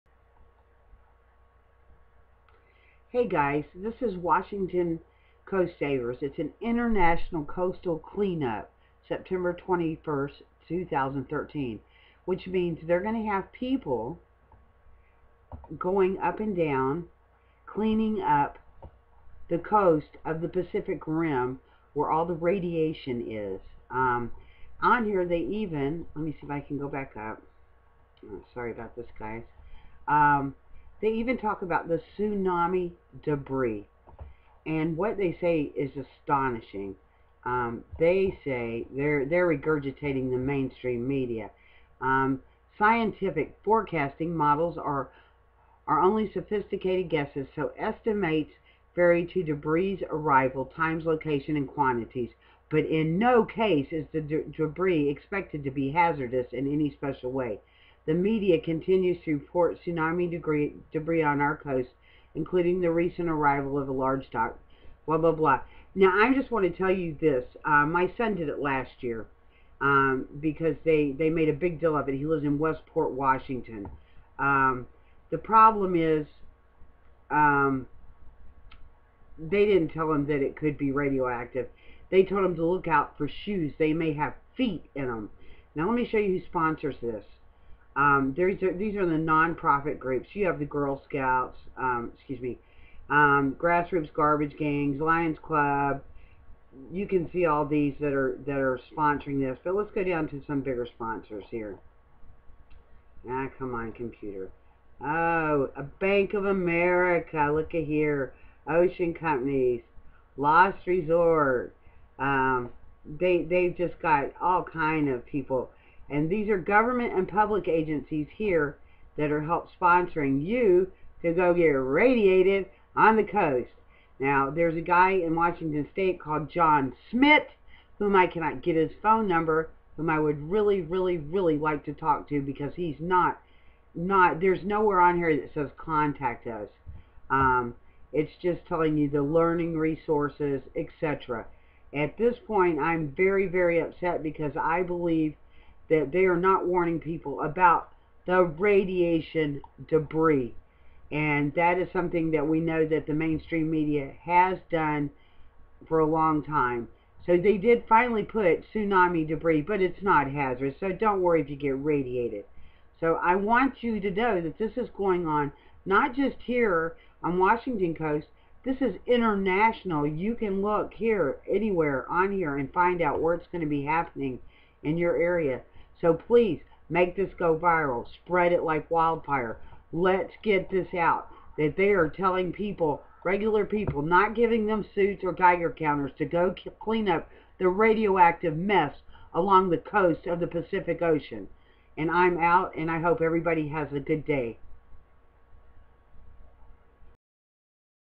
International Radiation cleanup day along the coast
These people are Lying and ENOUGH IS ENOUGH, My son did this last year and I told the commisioner off but my voice is not loud enough please help me scream about the radiation along the pacific coast Peace Love and Truth Linda